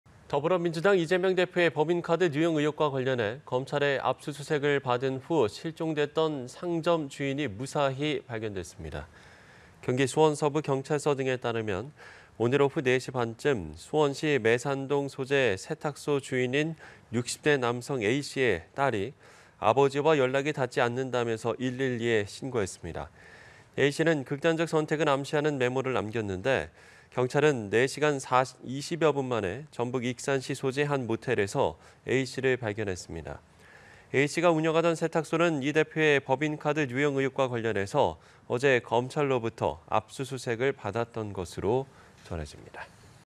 이재명 '법카 의혹' 압수수색 후 실종 상인…무사해 / 연합뉴스TV (YonhapnewsTV)
이재명 '법카 의혹' 압수수색 후 실종 상인…무사해

더불어민주당 이재명 대표의 법인카드 유용 의혹과 관련해 검찰의 압수수색을 받은 후 실종됐던 상점 주인이 무사히 발견됐습니다.

경기 수원서부경찰서 등에 따르면, 오늘(5일) 오후 4시 반쯤, 수원시 매산동 소재 세탁소 주인인 60대 남성 A씨의 딸이 "아버지와 연락이 닿지 않는다"며 112에 신고했습니다.

A씨는 극단적 선택을 암시하는 메모를 남겼는데, 경찰은 4시간 20여 분 만에 전북 익산시 소재의 한 모텔에서 A씨를 발견했습니다.

A씨가 운영하던 세탁소는 이 대표의 법인카드 유용 의혹과 관련해 어제(4일) 검찰로부터 압수수색을 받았던 것으로 전해졌습니다.